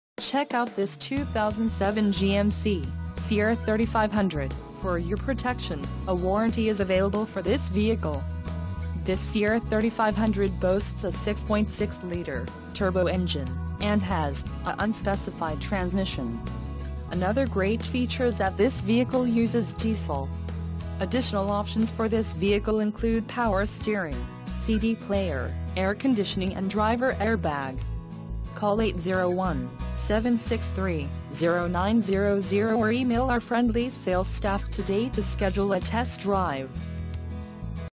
2007 GMC Sierra 3500 SLT Truck - American Fork, UT
Watts Automotive, American Fork, UT - 2007 GMC 3500 CREW CAB LONGBED DUALLY SLT 4X4 DURAMAX DIESEL LEATHER DVD